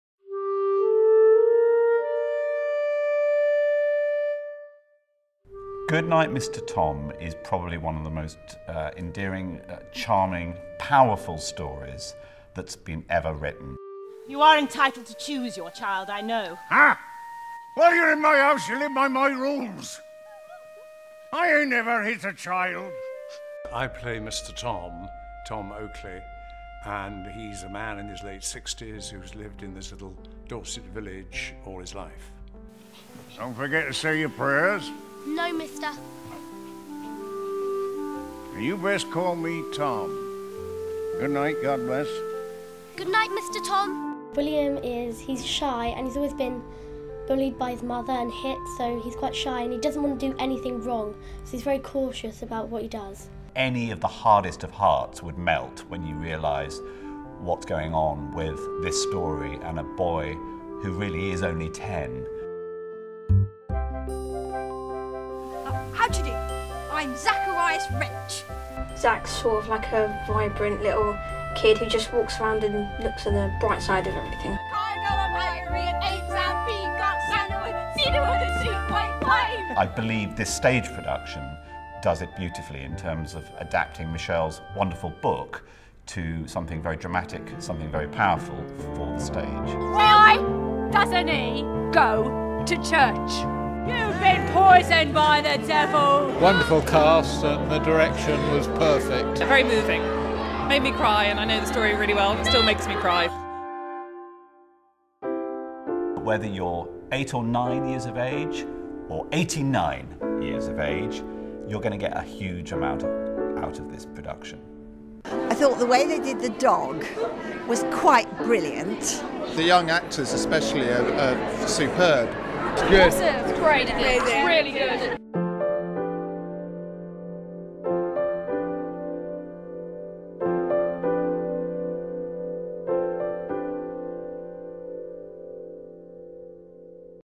Goodnight Mister Tom Live on Stage Trailer
Check out this trailer for the stage production of Goodnight Mister Tom at the Pheonix Theatre from 22 Nov to 26 Jan